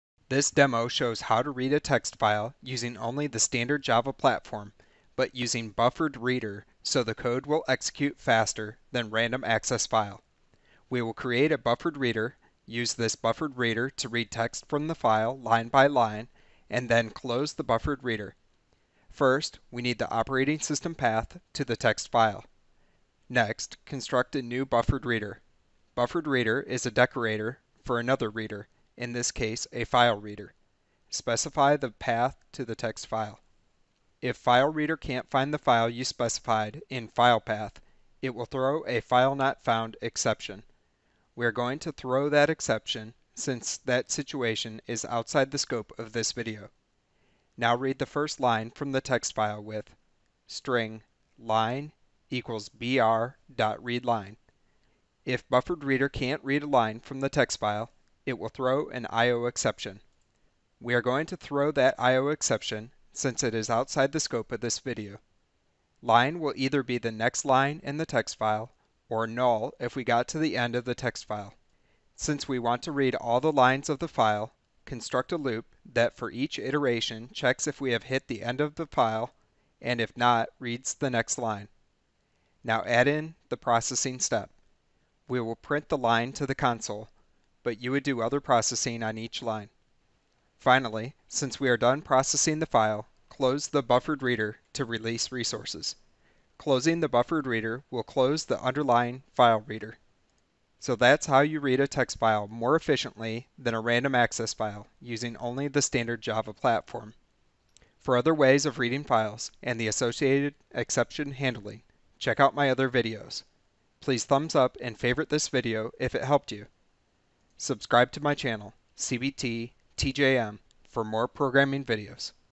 Java: Read text file efficiently with BufferedReader
Read a text file with standard Java BufferedReader. This is faster than RandomAccessFile, but slightly more lines of code. Demo of BufferedReader and FileReader. We process the text file line by line and then close the text file. Good beginning programming example. Follow along!